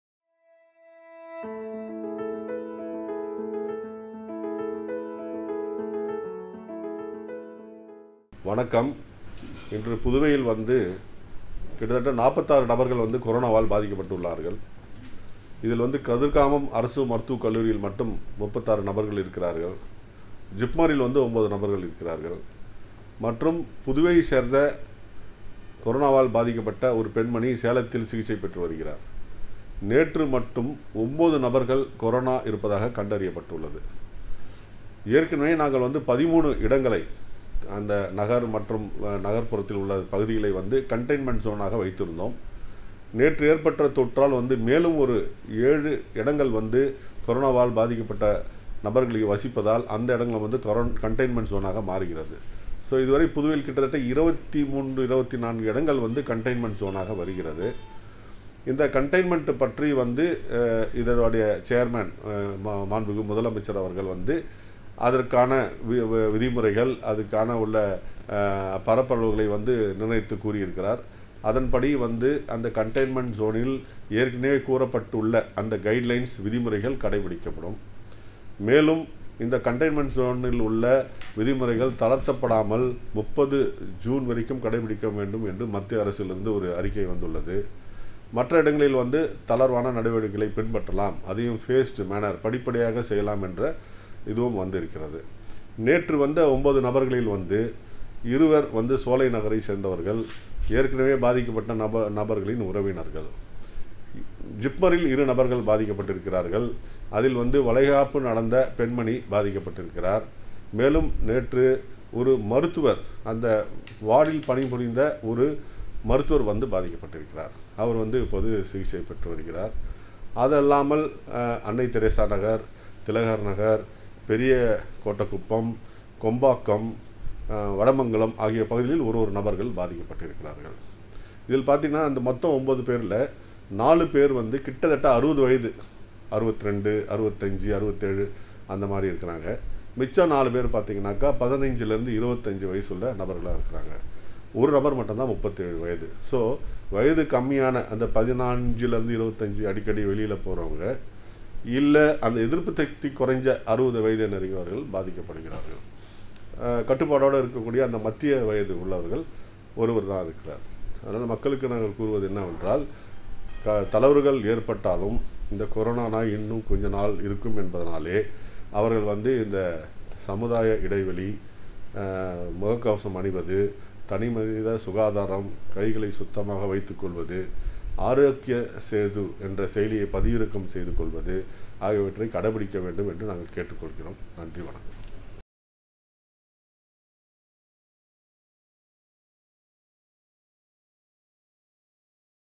Press meet by Health Director Dr. S. Mohan Kumar on COVID -19 Updates in Puducherry [31-05-2020]
"This video is presented by Doordarshan Kendra, Puducherry"